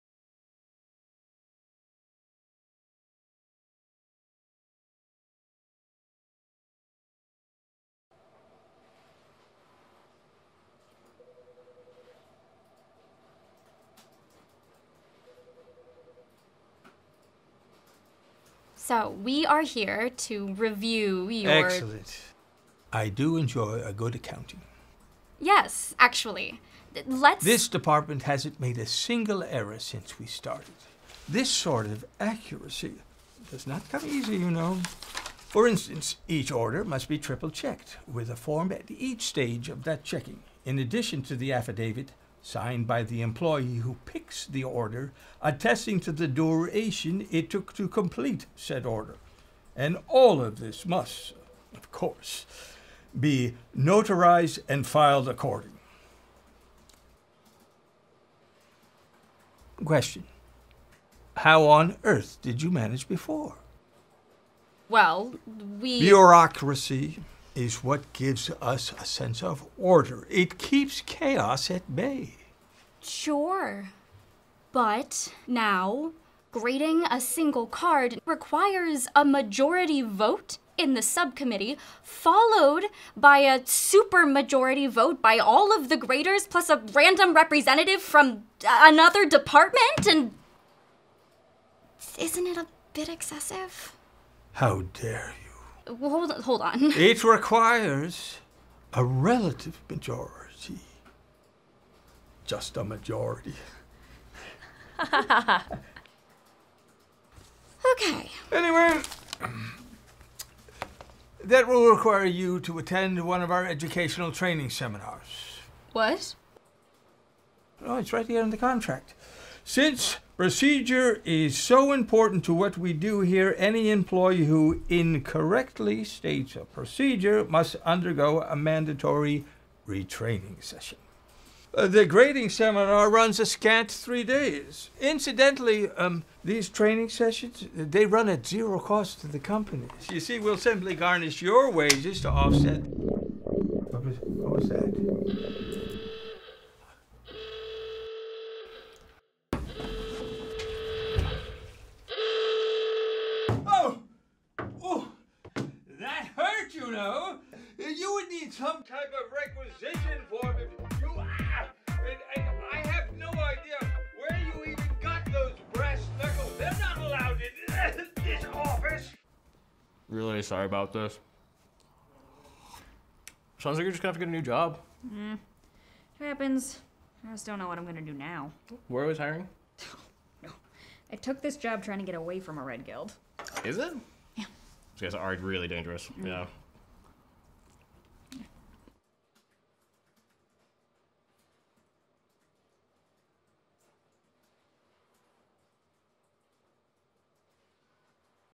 Azorius Takes Over Card Kingdom | Magic: the Gathering | Ravnica Allegiance Comedy Short
After firing the Izzet League, we've hired the Azorius to provide some stability in our office.

Here at Card Kingdom, we're always looking for ways to be better and faster. Back in the fall, we brought in a consultant from the Izzet League to help us improve operations in our fulfillment center. But when their Machine unleashed an Inescapable Blaze, we thought we could use a change of pace.

Now, we've turned to the Azorius Senate to promote order and stability in our office. But subcommittees and majority votes on card condition weren't exactly what we were looking for. Sometimes, when you're bogged down in red tape, you have no choice but to go aggro.